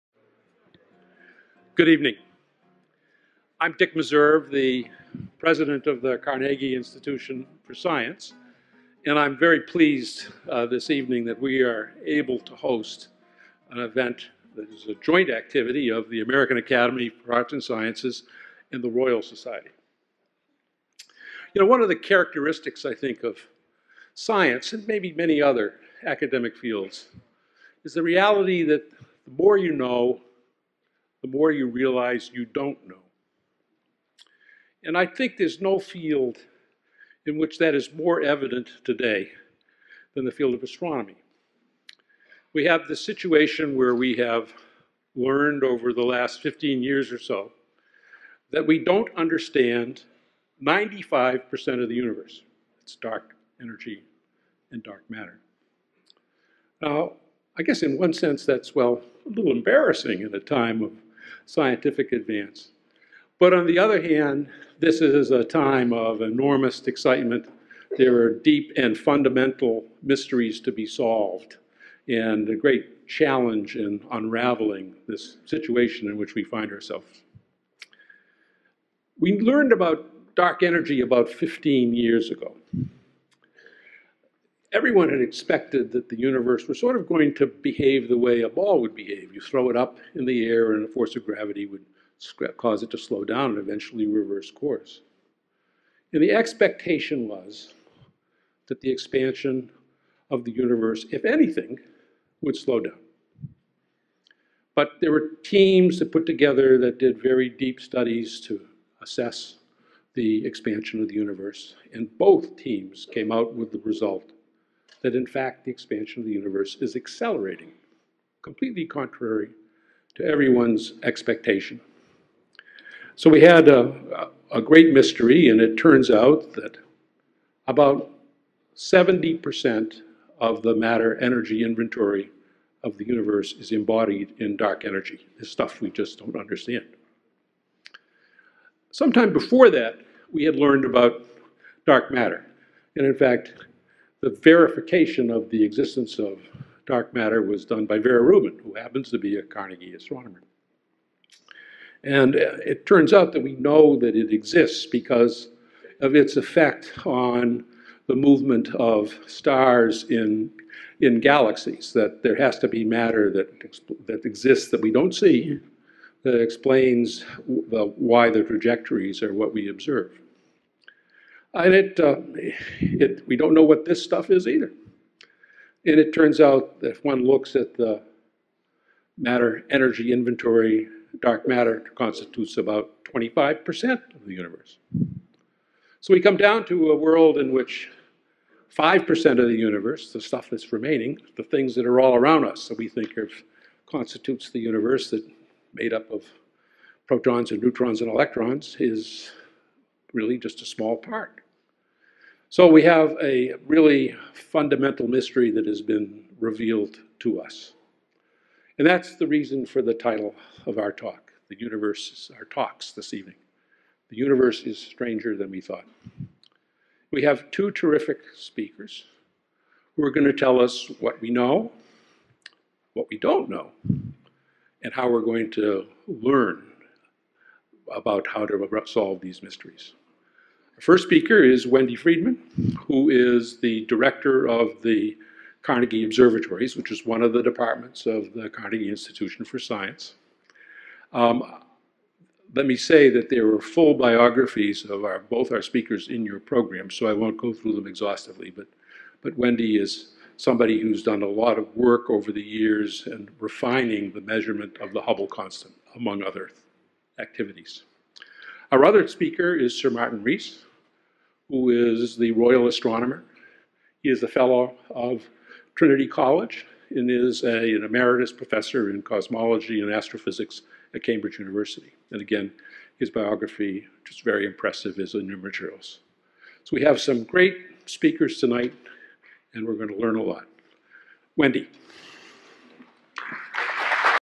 Richard A. Meserve - Welcome and Call to Order
The Universe is Stranger Than We Thought
April 29, 2014
Carnegie Institution for Science
Washington, D.C.

American Academy of Arts and Sciences